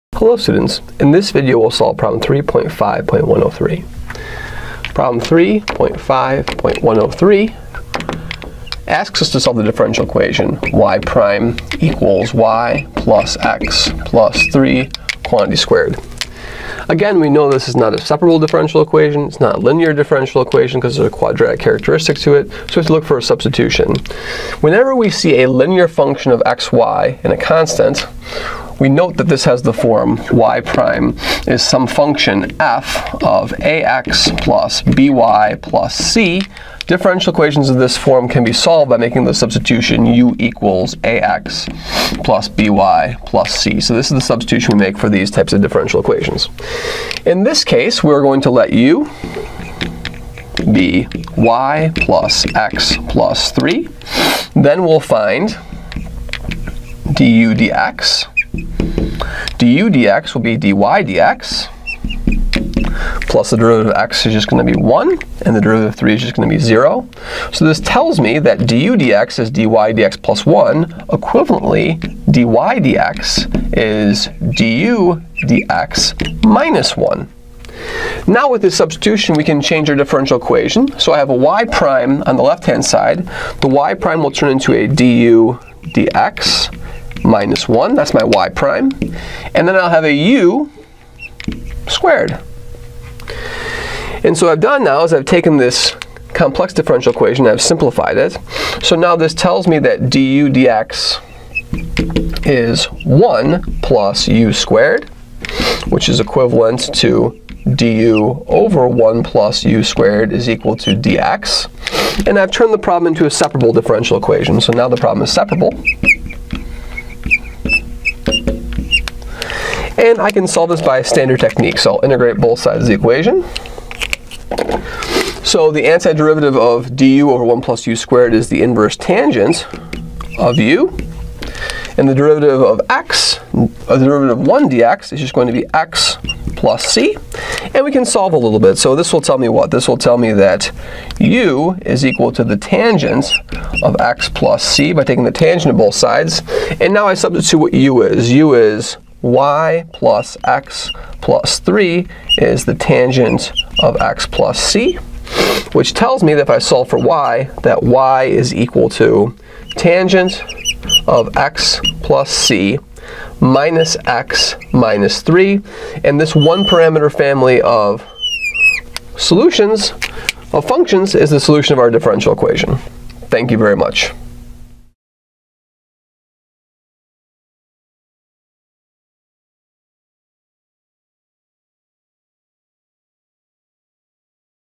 Solving a First Order ODE by Substitution An Example
We give an example of solving a first order ODE by making a apparent substitution.